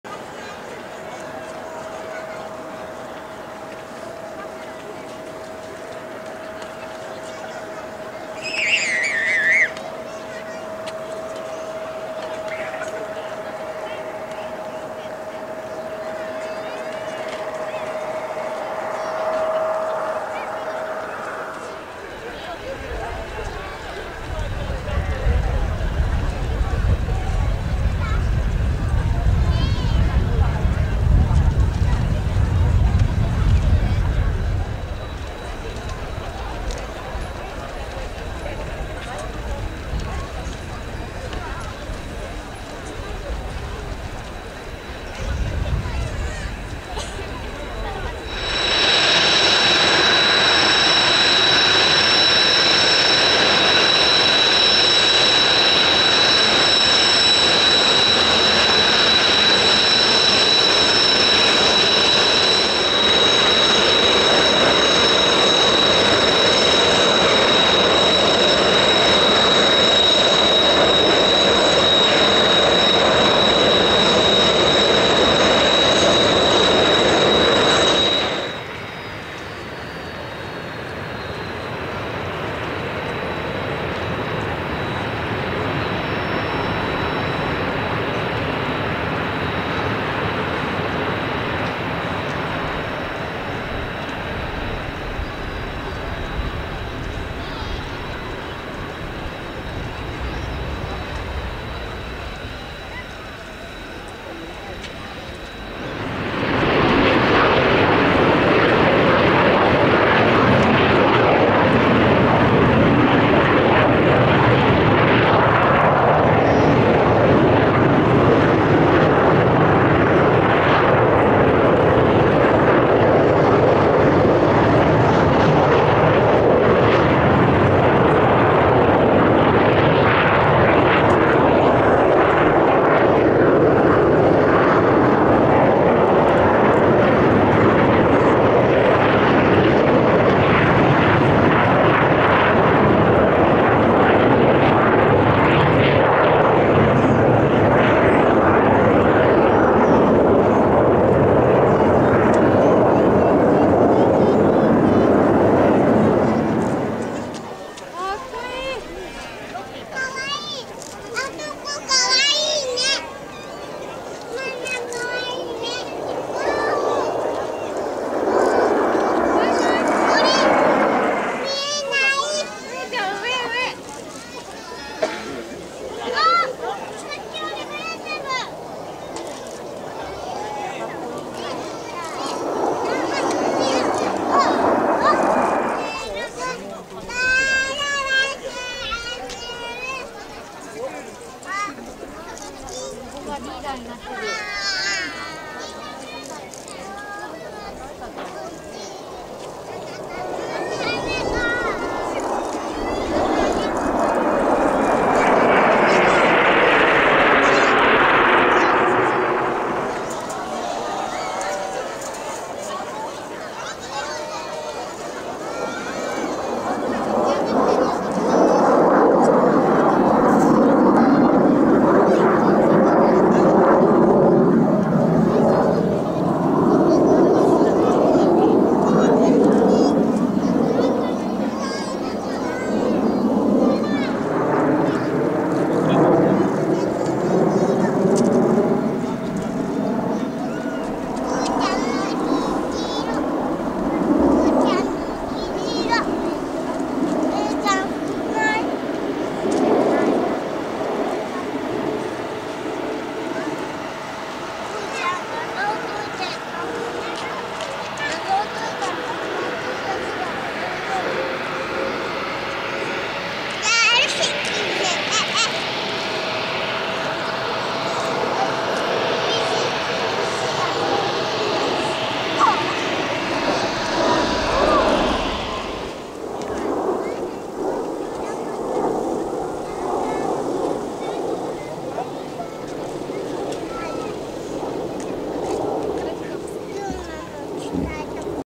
航空祭の花はブルーインパルス（小牧基地航空祭2023）
やっぱりブルーはカッコいいよね～航空祭を華やかにしてくれるスターですね、
他の航空祭で見る事が出来ると思いますが楽しみにしてます、
ありがとうブルーインパルス！